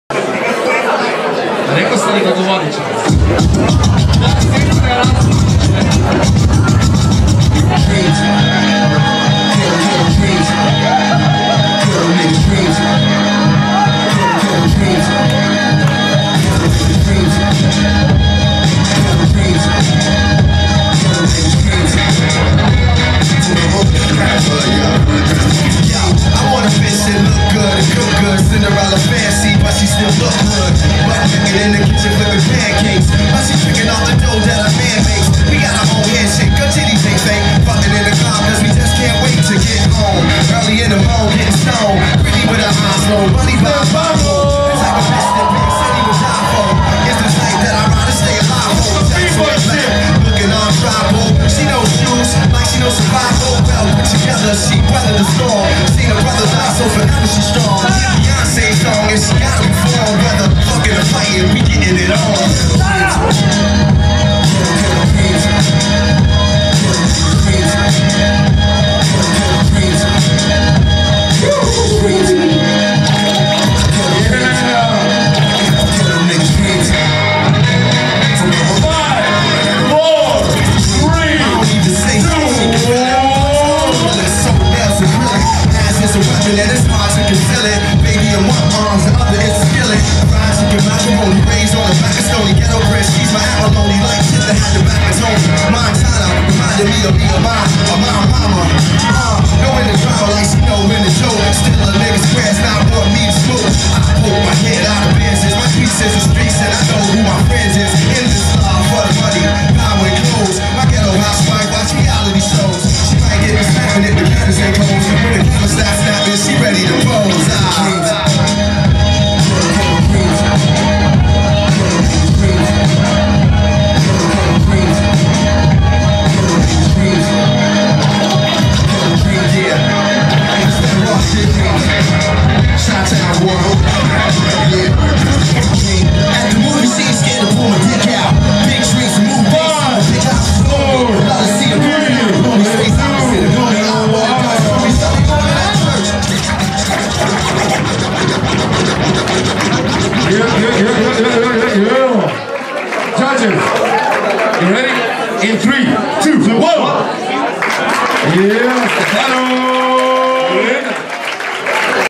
STEFANO vs STELA/Spread The Culture – Siget battles Vol.2 (hip hop/top 32)
Spread The Culture - Siget battles Vol.2
Hip hop battles 1 vs 1 (top 32)
Judges: Edwin Batalla, Joana Hormann, Mulla Zuev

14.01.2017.
Zagreb, Croatia